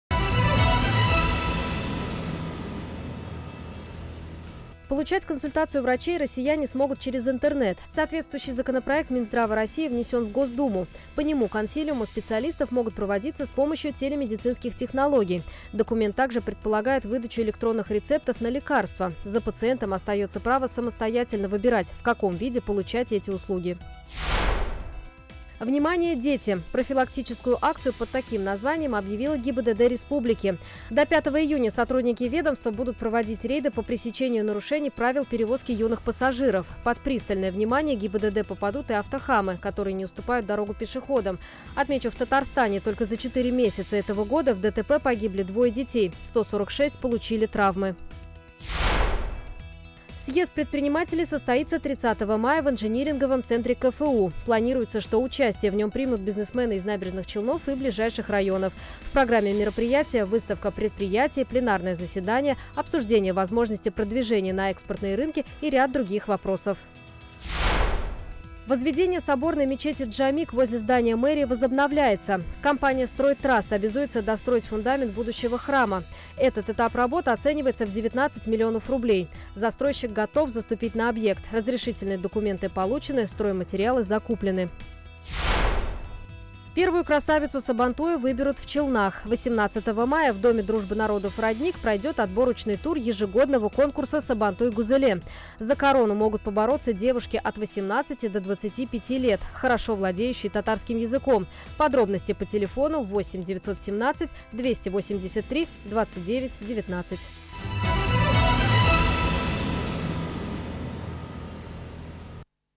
КОРОТКО И ЯСНО
Получать консультацию врачей россияне смогут через Интернет. Соответствующий законопроект Минздрава России внесен в Госдуму. По нему консилиумы специалистов могут проводиться с помощью телемедицинских технологий.